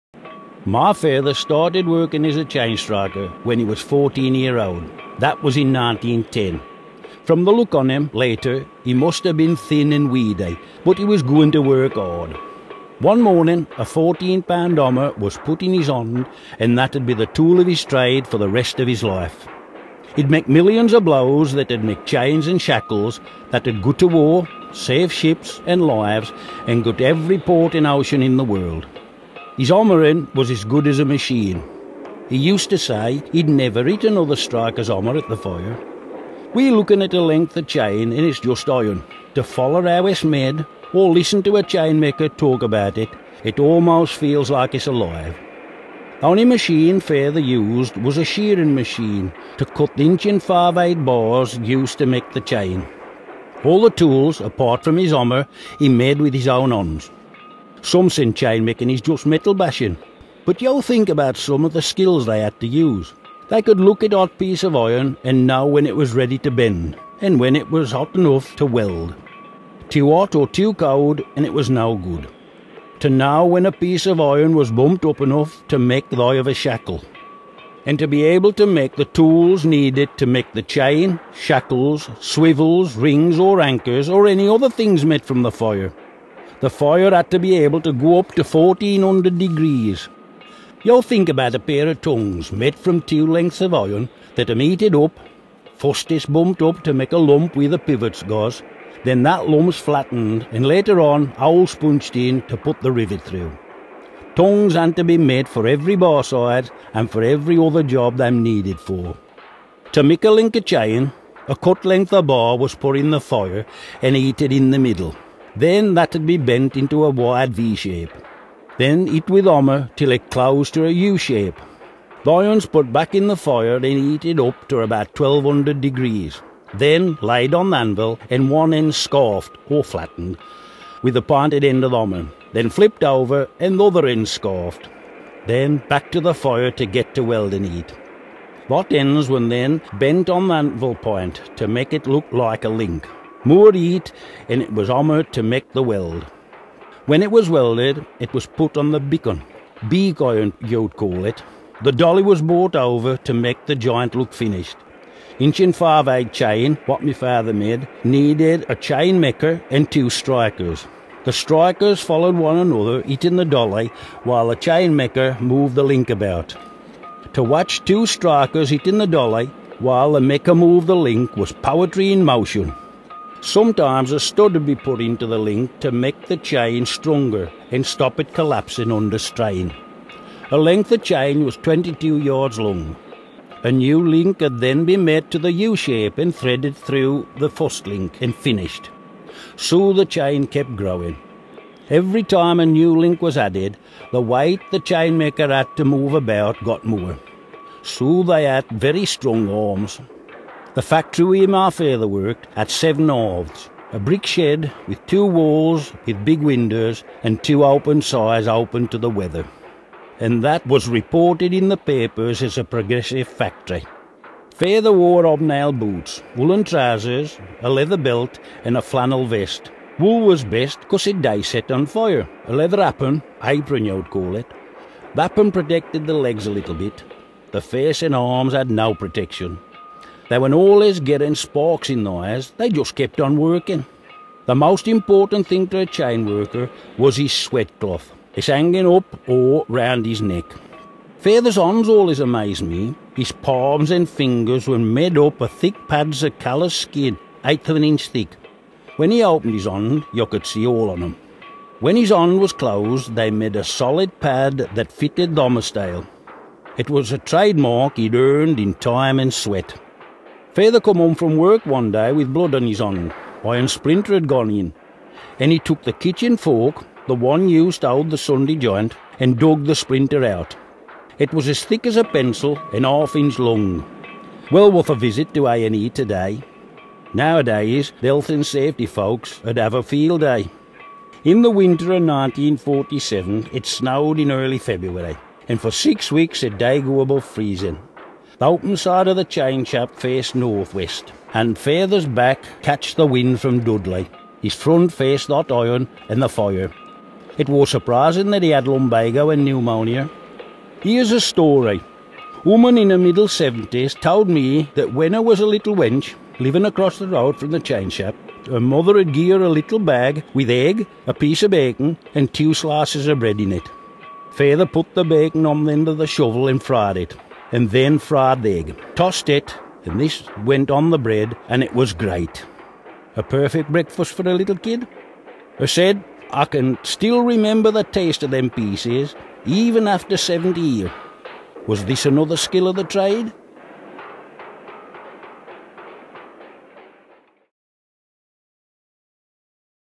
Chainstriker
Short film telling a story of a chainstriker from Cradley named Clifford Willetts. Narrated in the local Black Country dialect by his son Barry. This short is part of the 6 hour long short film collection entitled Cradley Then & Now, available